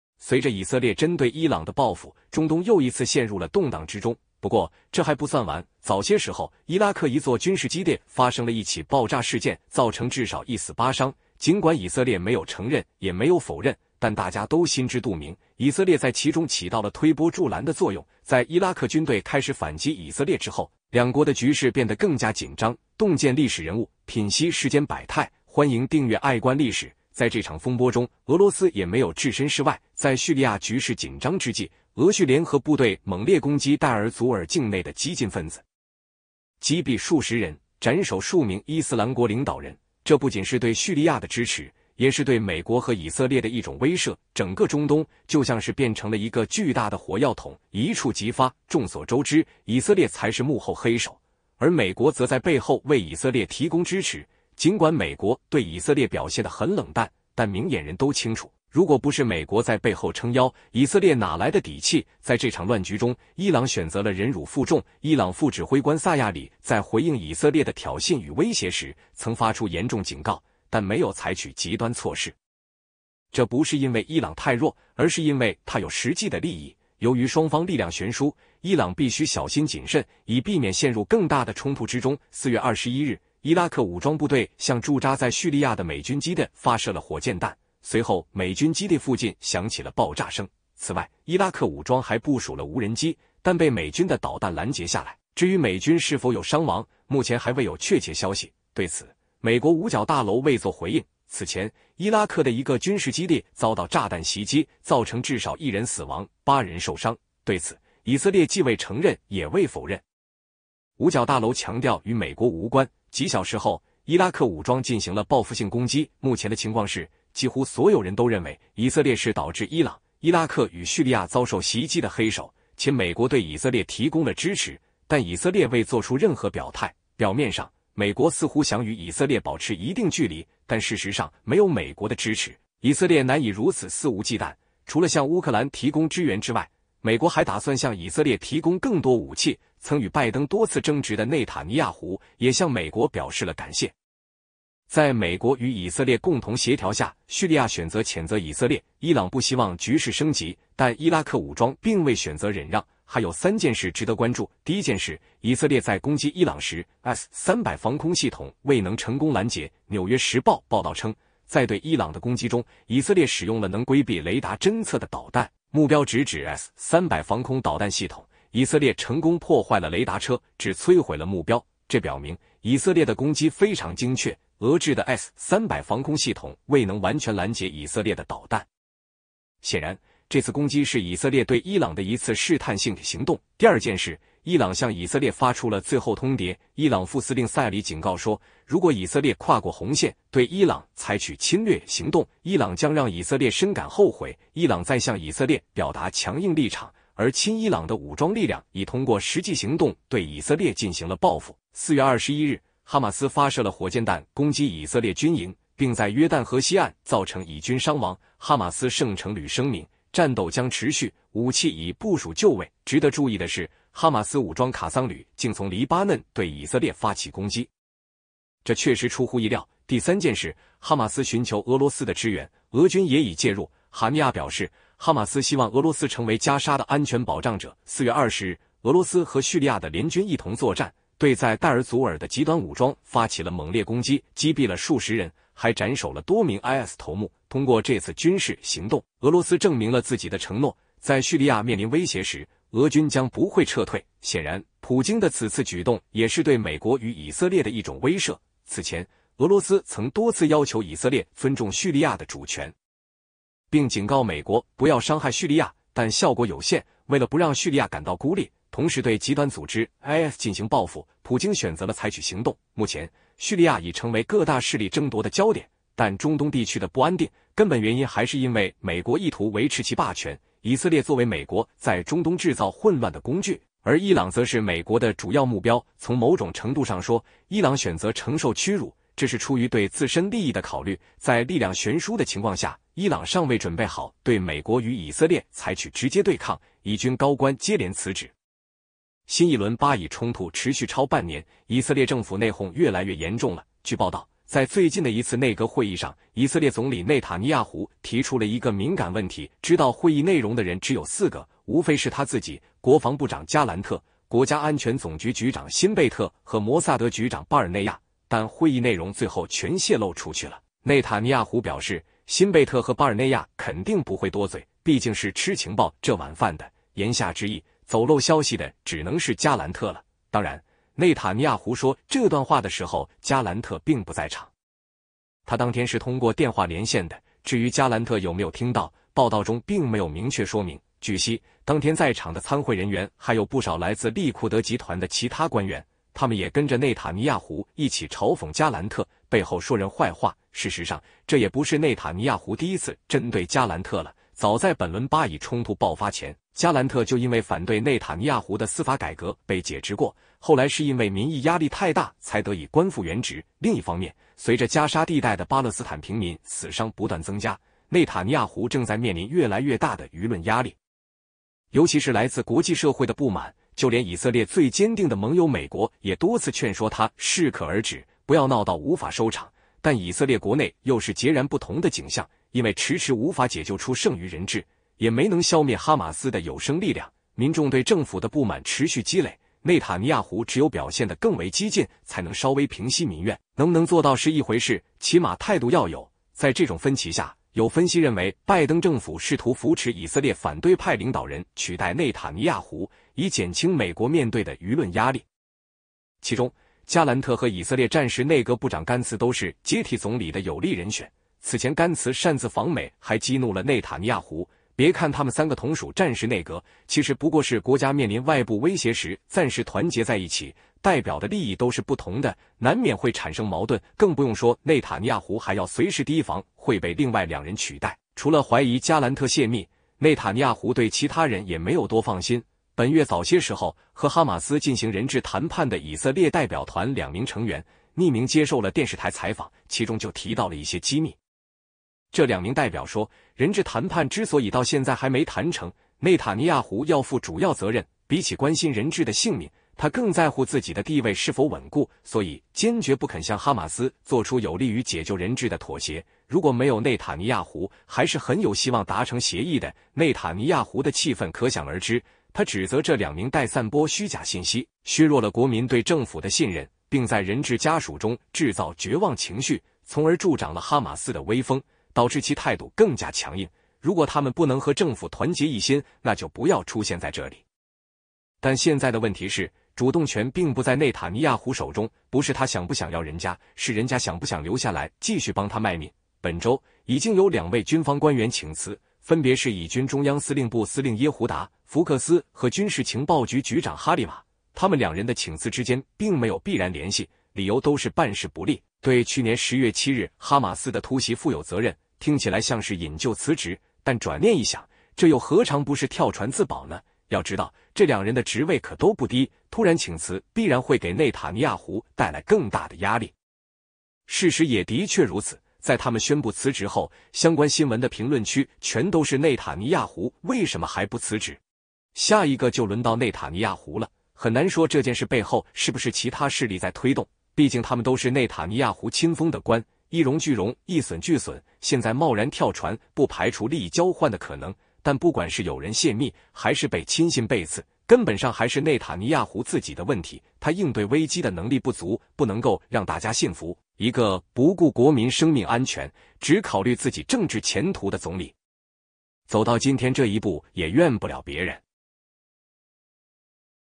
又打起來了，以色列多地被炸，美軍基地遭炮轟，俄軍也出手了【愛觀歷史】歷史
隨著以色列針對伊朗的報復，中東又一次陷入了動蕩之中。不過，這還不算完。早些時候，伊拉克一座軍事基地發生了一起爆炸事件，造成至少1死8傷。盡管以色列沒有承認，也沒有否認，但大家都心知肚明，以色列在其中起到了推波助瀾的作用。在伊拉克軍隊開始反擊以色列之後，兩國的局勢變得更加緊張。